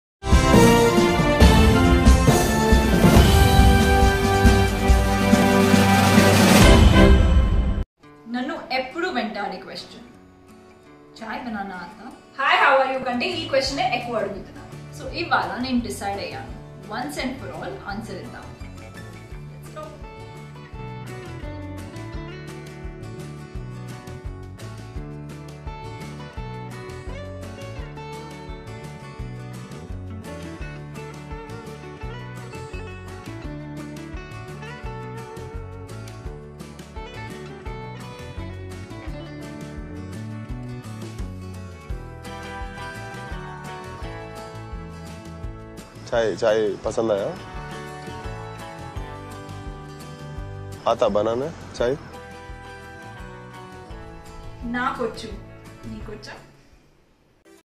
Actress Simran Choudhary cooking video | Simran Choudary Making Tea Video | Filmy Clicks
Watch Latest Simran Simranchoudhary videos here